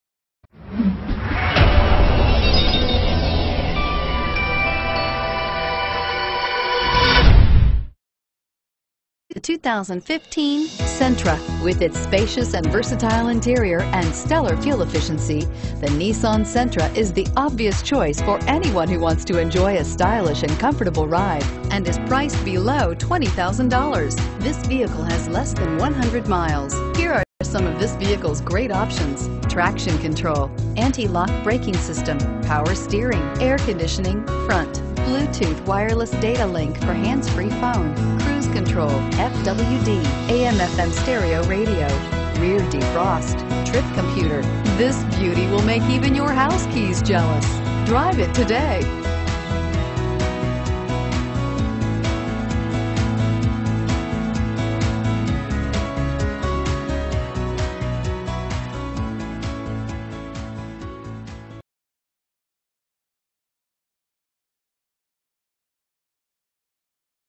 2015 Nissan Sentra Newnan GA Y233562
2015 Nissan Sentra S

Don't miss this 2015 Nissan Sentra. It's equipped with Automatic transmission and features a Super Black exterior. With 5 miles, You'll want to take this car home. Make a great choice today.